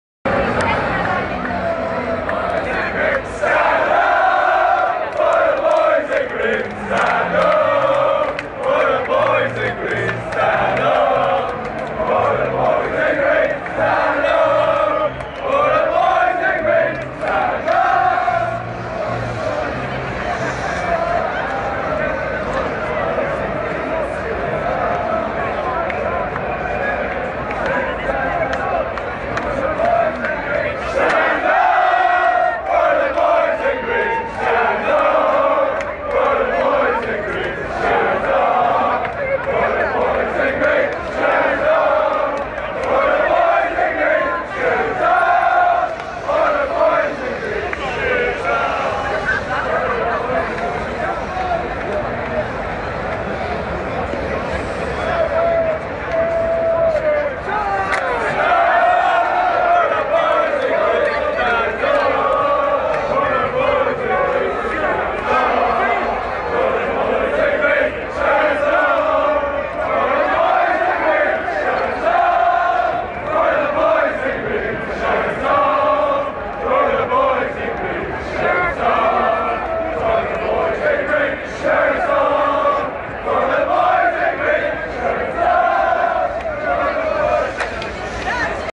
KIBOLE IRLANDCY NA STARYM RYNKU W POZNANIU!!!
PRZED MECZEM IRLANDIA I CHORWACJA!!!